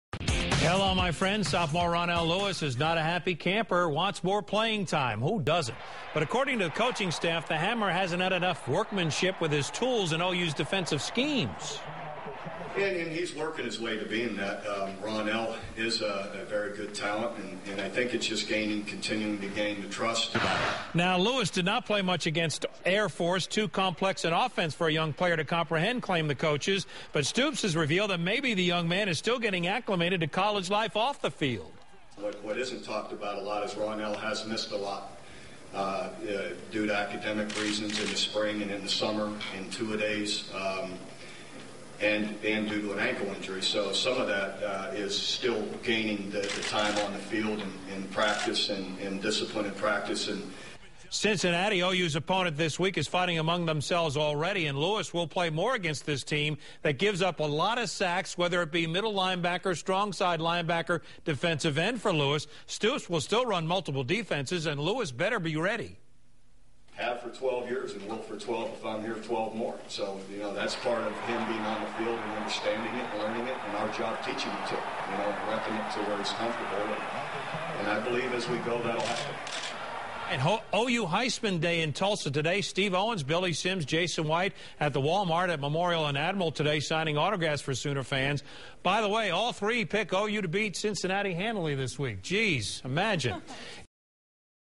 Ronnell Lewis wants more playing time
Stoops says Lewis has missed too many workouts